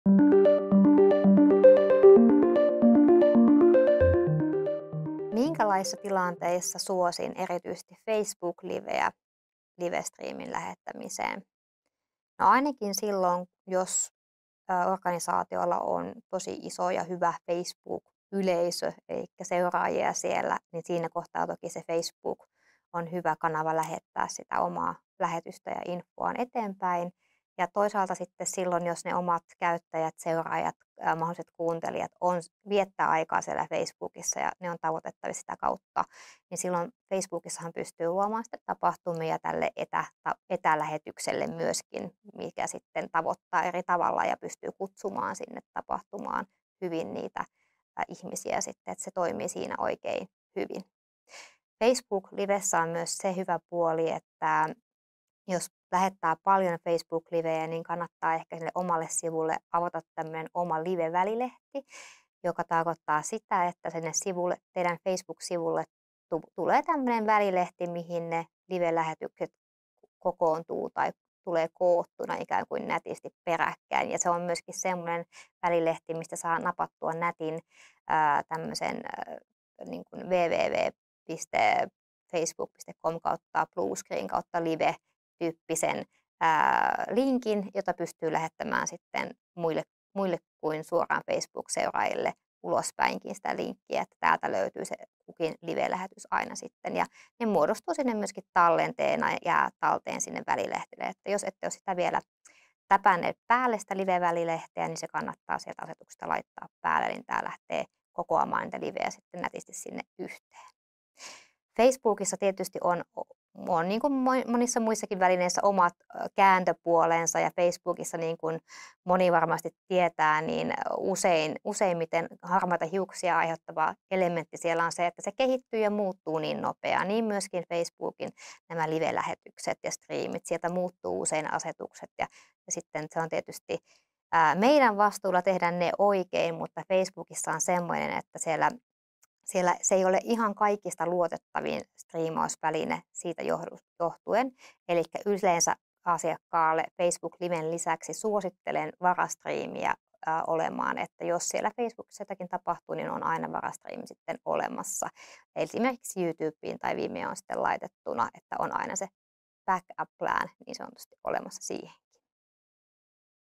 UKK - Mihin Facebook Live sopii parhaiten?
Milloin Facebook Live soveltuu live-lähetyksen jakelukanavaksi? Miten voit hyödyntää Facebookin live-työkalua paremmin? Entä miten toimit, jos Facebookin Liven kanssa tulee ongelmia? Vastauksia näihin kysymyksiin tällä videolla!